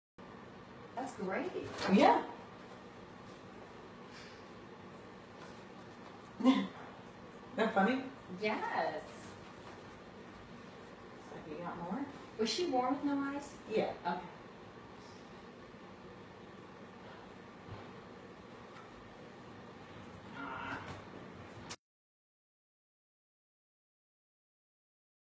Cordelia, born without eyes and deaf, shows off her tricks!
Miracles happen every day at the Pet Alliance! Cordelia was born without eyes and is completely deaf but it has not kept her from loving life. Watch as she shows off her new tricks for yummy treats!